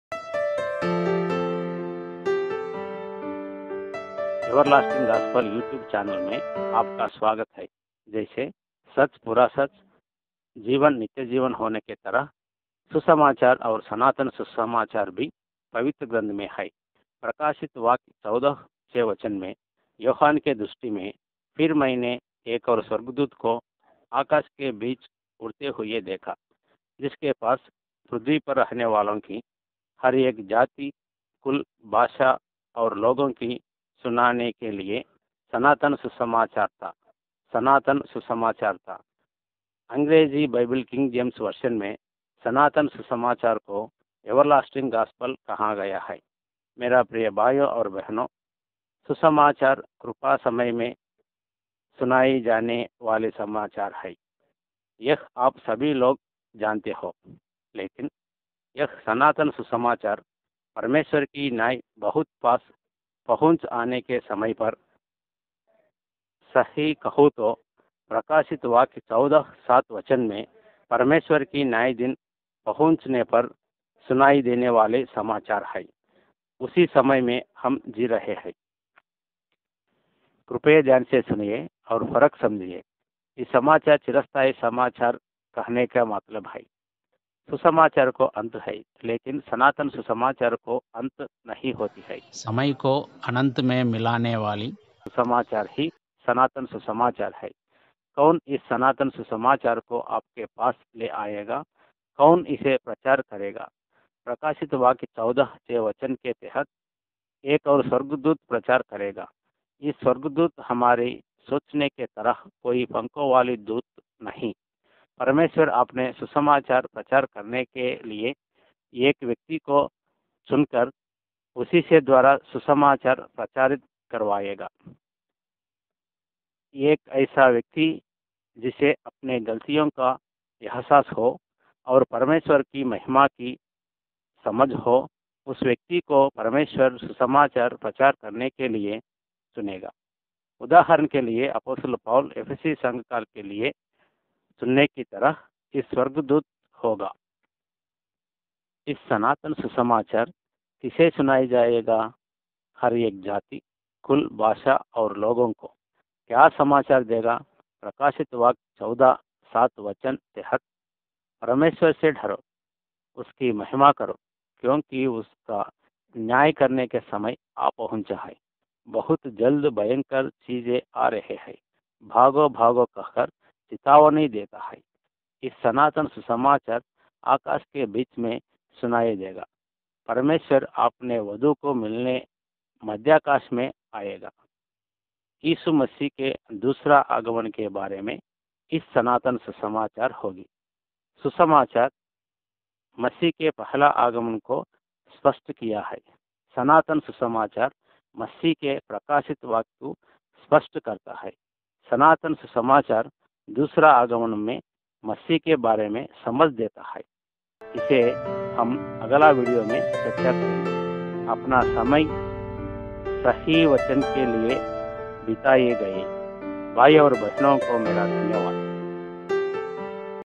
1.परमेश्वर के दुल्हन को समाचार . Everlasting Gospel to the BRIDE of CHRIST.
Everlasting Gospel YouTube channel आप का स्वागत है ।जैसे सच - पूरा सच, जीवन - नित्य जीवन, होने की तरह, सुसमाचार और सनातन सुसमाचार भी पवित्र ग्रंथ में है ।
*प्रकाशित वाक्य 14 : 6 वचन में जोहन के दृष्टि में , फिर मैंने एक और स्वर्ग दूत को आकश के बीच उड़ते हुए देखा, जिसके पास , प्रुधवी पर रहने वालों की हार एक जाति कुल भाषा और लोगों को सुनाने के लिए सनातन सुसमाचार था।
प्रकाशितवाक्य 14:6
फिर मैंने एक और स्वर्गदूत को आकाश के बीच में उड़ते हुए देखा जिसके पास पृथ्वी पर के रहनेवालों की हर एक जाति, कुल, भाषा, और लोगों को सुनाने के लिये सनातन सुसमाचार था।
* अंग्रेजी बाइबल king james version में सनातन सुसमाचार को everlasting gospel कहा गया  है। मेरे प्रिय भाईयो और बहनों सुसमाचार कृपा समय में सुनाए जाने वाली समाचार है। यह आप सभी लोग जानते हो लेक यह सनातन सुसमाचार परमेश्वर की न्याय बहुत पार पहुंचने समय पर, सही काहू तो प्रकाशित वाक्य 14: 7 वचन के तहत  परमेश्वर के न्याय पहुंचने पर सुनाई जाने वाली समाचार है।
*  और उसने बड़े शब्द से कहा, “परमेश्वर से डरो, और उसकी महिमा करो, क्योंकि उसके न्याय करने का समय आ पहुँचा है; और उसकी आराधना करो, जिसने स्वर्ग और पृथ्वी और समुद्र और जल के सोते बनाए।”
*  उसी समय में हम की रहे है। क्रियाएं धयन से सुनिए और फरक समझिए, इस समाचार चिरस्ताय समाचार कहने का मतलब है, सुसमाचार को अंत है लेकिन सनातन सुसमाचार को अंत नहीं होगा। समय को अनंत में मिलने वाले समाचार ही सनातन सुसमाचार है।
* कौन इस सनातन सुसमाचार को आप के पास ले आए गा, कौन इसे प्रचार करेगा, प्रकाशित वाक्य 14:6 वचन के तहत एक और स्वर्ग दूत , प्रचार करेगा।
*  इस स्वर्ग ड्डोथ हमारी सोच की तरह कोई पंखों वाली  दूत नहीं। परमेश्वर अपने सुसमाचार प्रचार करने केलिए, एक व्यक्ति को चू कर उसी के द्वारा सुसमाचार प्रचार करवाए ग।* एक ऐसा व्यक्ति जिसे अपने गलतियों का एहसास हो , और परमेश्वर की महिमा की समाज हो , उस व्यक्ति को परमेश्वर अपने सुसमाचार प्रकट करने केलिए चुनेगा।
 *  उदाहरण के लिए अपोस्टेल पौलूस एफेयसी संघ  के लिए चुनने की तरह इस  स्वर्ग दूत होगा।
* इस सनातन सुसमाचार किसे सुनाए जाएगा? हर एक जाति, कुल , भाषा, और लोगों को।